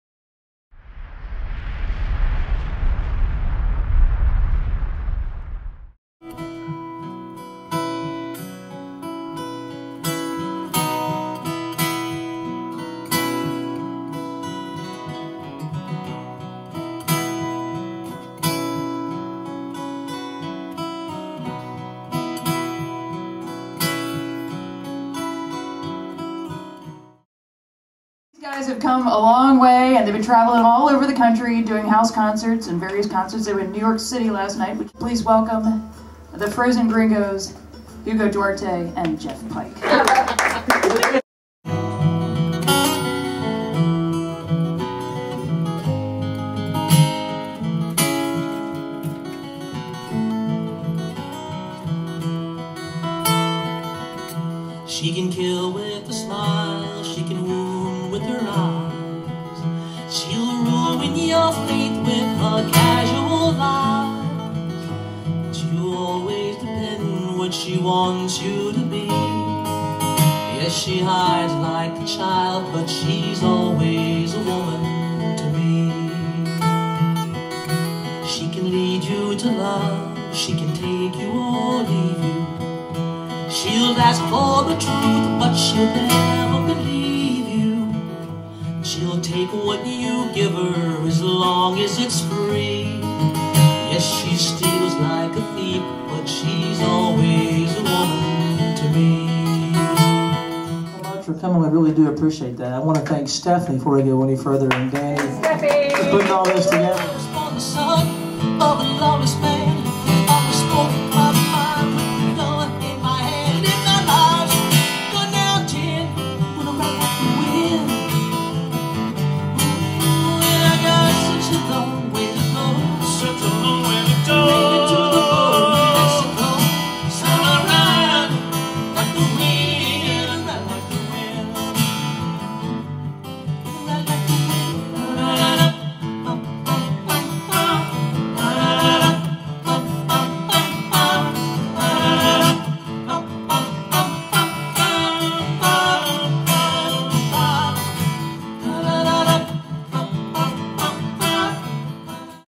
Hugo Duarte and Jeff Pike of the Frozen Gringos on the BADOG MUSIC Community Stage
Hugo Duarte and Jeff Pike of the Frozen Gringos perform on the BADOG MUSIC Community Stage. For more information on upcoming shows, go